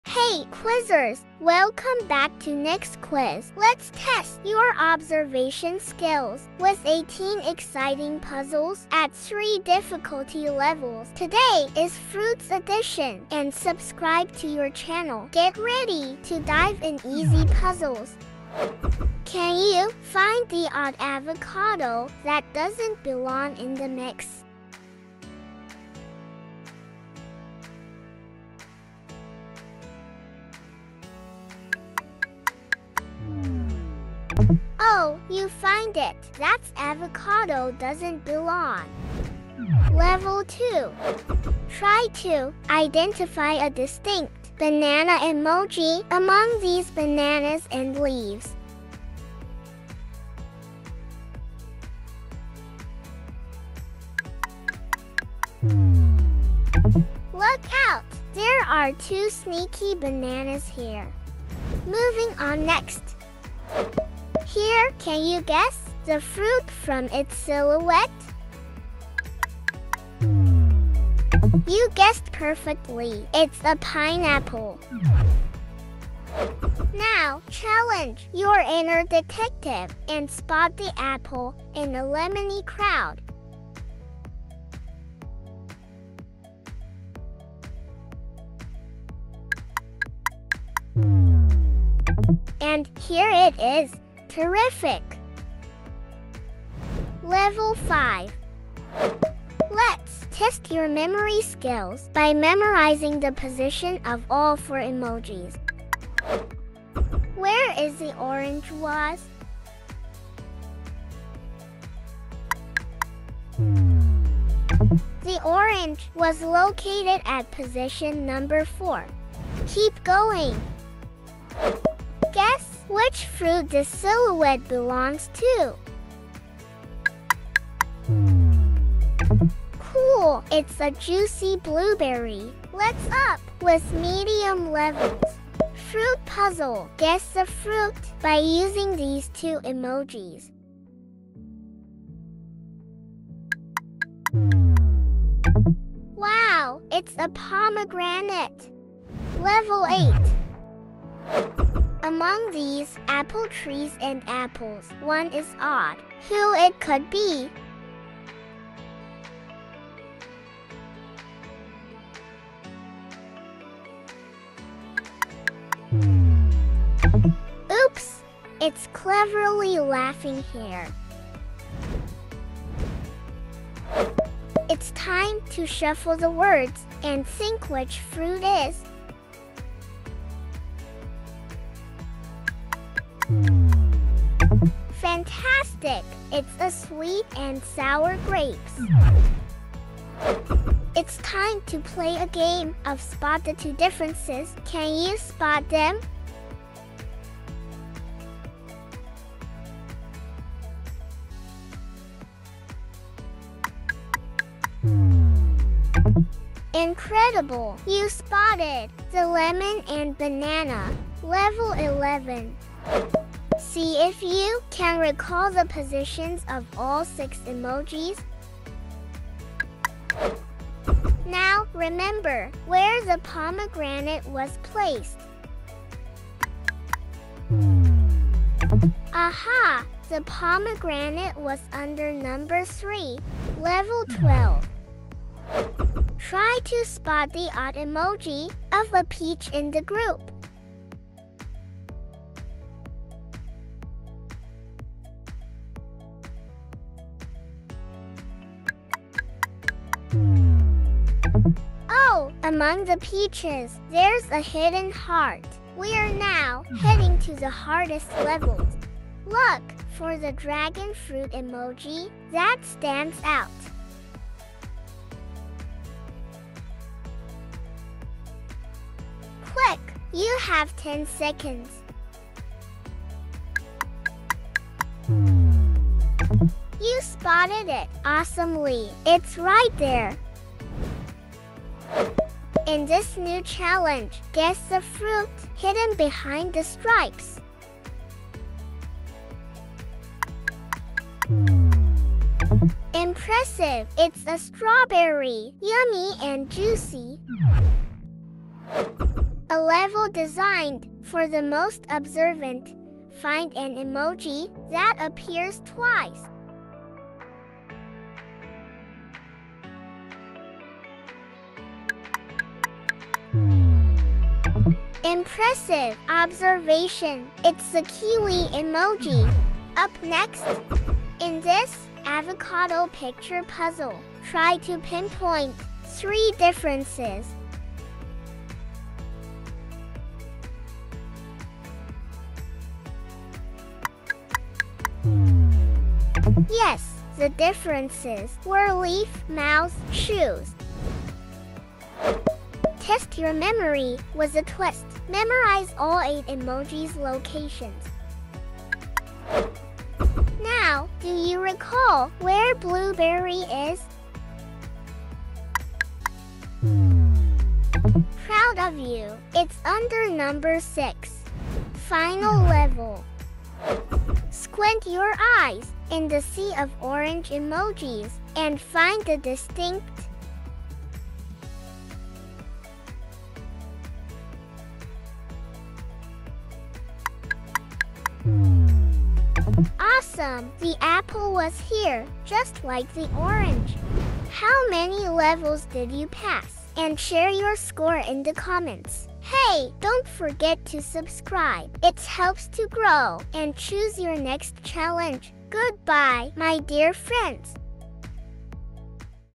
Find the ODD One Out - Fruit Edition 🍇🥑🍒 Easy, Medium, Hard - 18 Ultimate Levels Emoji Quiz
Join us in our ultimate fruit challenge with an exciting new video quiz! 🍒🎉 Can you find the odd fruit emoji and ace our emoji challenge? Our quiz has different levels of difficulty, ranging from easy to epic, and is designed to put your observation skills to the test and push your powers of perception to the limit.

Whether you are trying to find the odd avocado among pears 🥑 or guess the fruit by emoji, each level promises a unique and engaging experience. This video is perfect for food quiz enthusiasts and anyone looking to test their observation skills. It's packed with fun, creativity, and a dash of healthy competition.

Join us on a journey through easy, medium, and hard levels and see how good your eyes are. Don't miss out on the ultimate test of your observation power. Play along, challenge your friends, and discover if you have what it takes to conquer our ultimate levels in the ultimate fruit challenge! 🍇🥑🍒